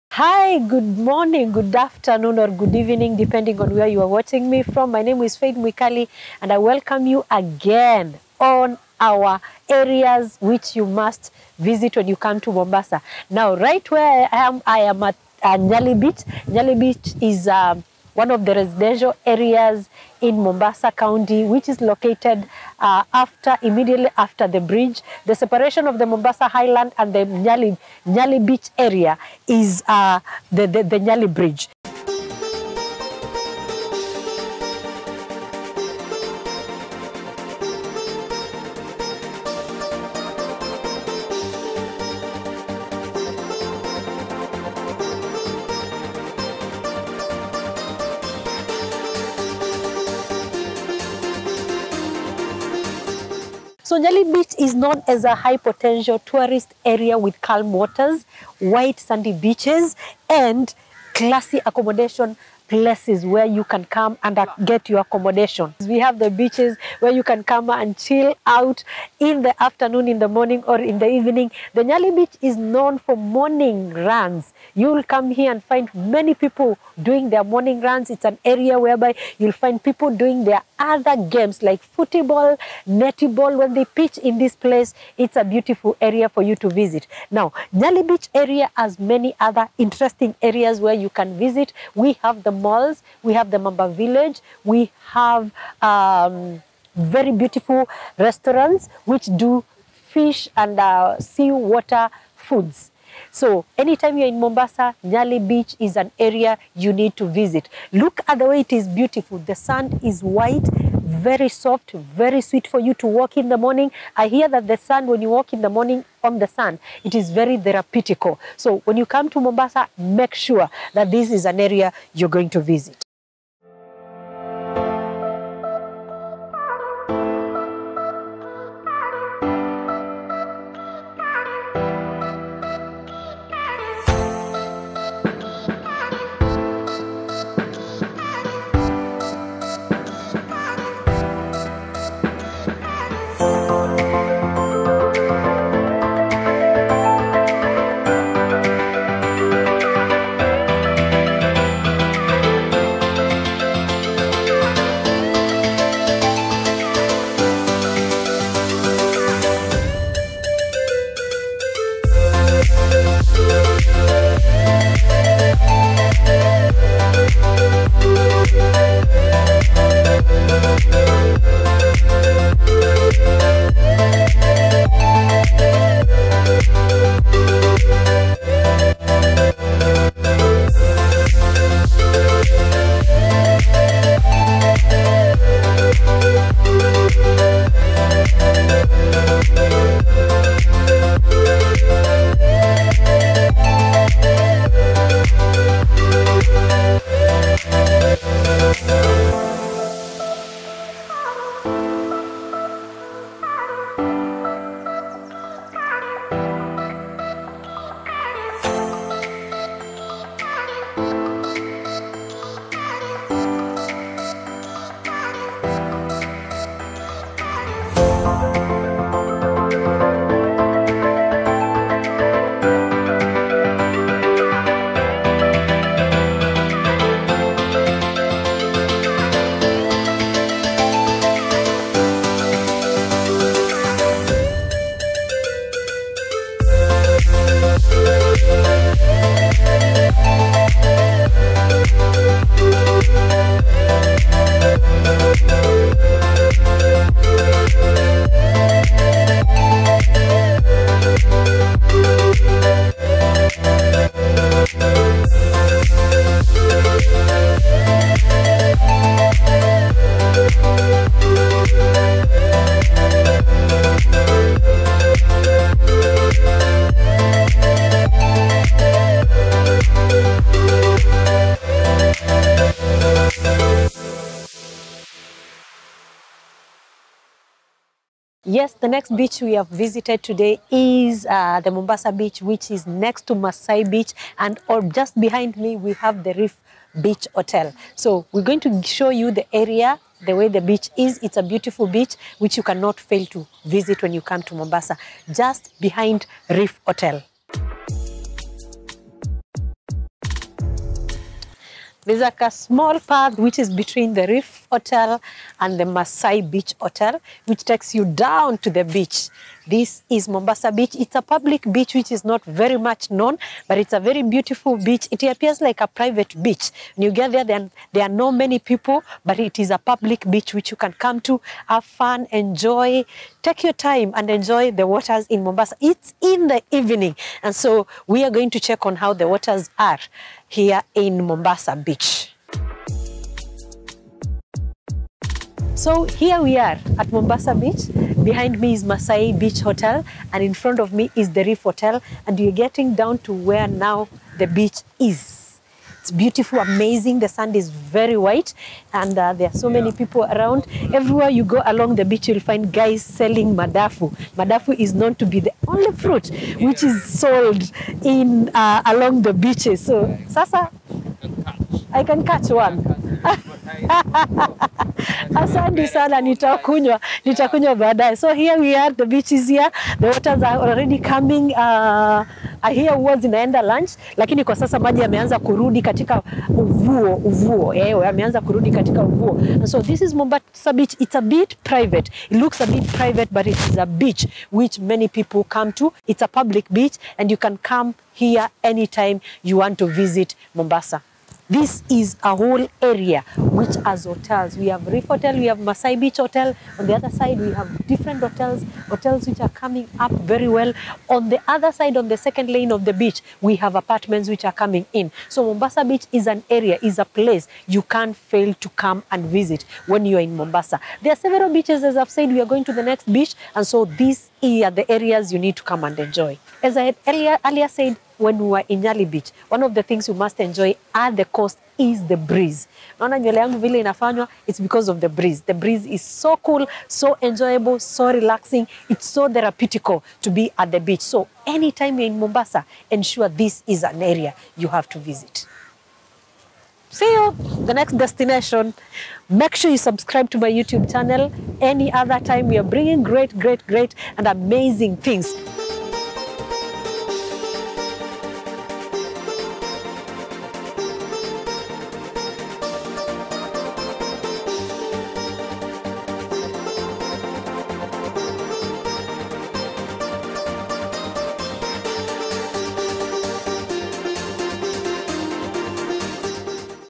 NYALI AND MOMBASA BEACH - AREAS TO VISIT WHILE IN MOMBASA WITH FAITH MWIKALI. LETS DO THE BEACHES,
Nyali and Mombasa beaches are inthe Northern Part of Mombasa county and they are beautiful for relaxing and enjoying a cool breeze.